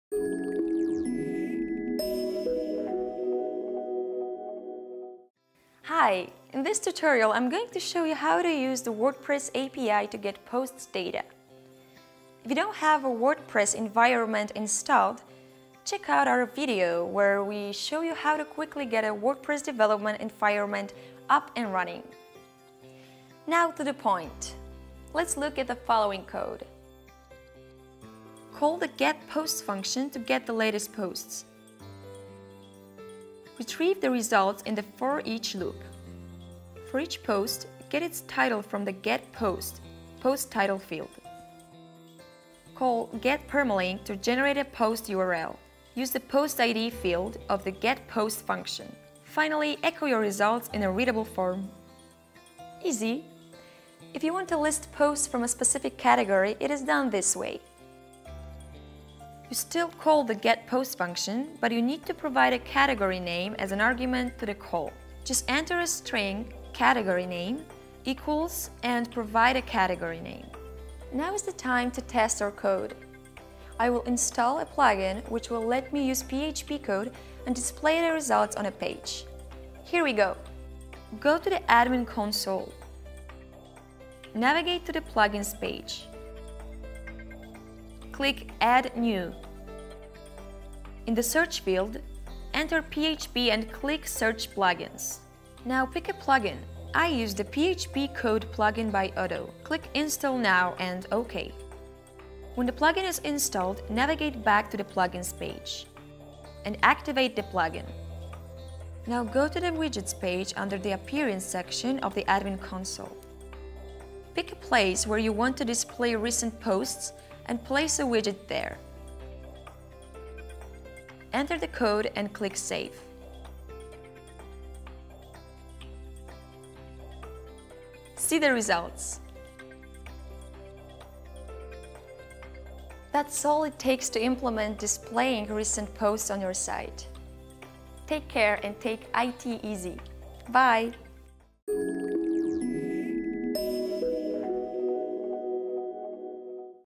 How to get posts using WordPress API
In this tutorial we are going to show you how to use WordPress API to get posts data. If You don't have a WordPress Environment installed check out our video where we show how to quickly get WordPress Development Environment up and running.